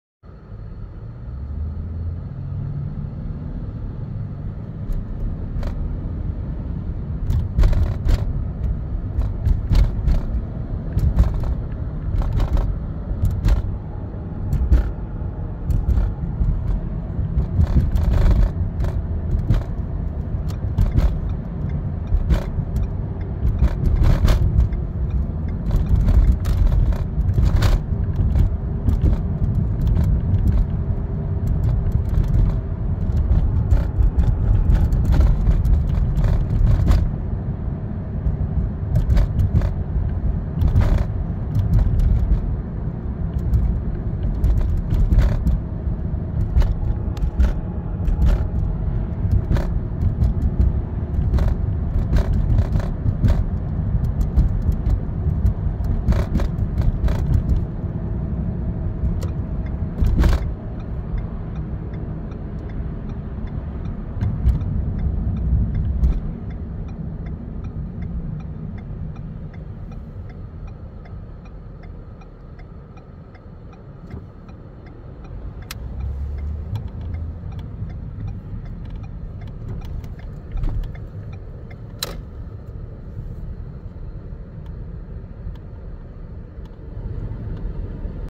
2024 (?) - 24/33❄️🐈🐈‍⬛🏡Kevs busy day feed cats, beach, injury hot tub, car wash, relax 30F 79% 15mph
Samsung S21 Ultra smartphone, Fri Nov 29